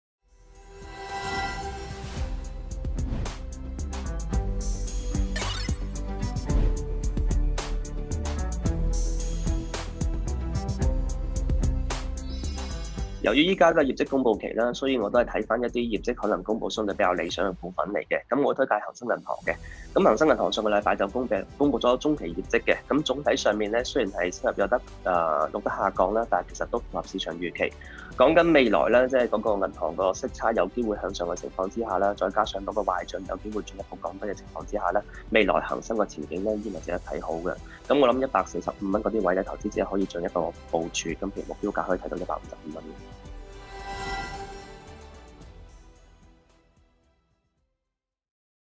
【潛力股精選】伍禮賢：恒生銀行（00011）
恒生銀行（00011）截至今年6月底止中期業績，純利87.7億元，按年減少4.1%。但集團增加派息，第二次中期息每股派1.1元，光大新鴻基證券策略師伍禮賢建議投資者可考慮買入恒生銀行，想知點解，立即點擊收聽或收看。（開聲聽↓）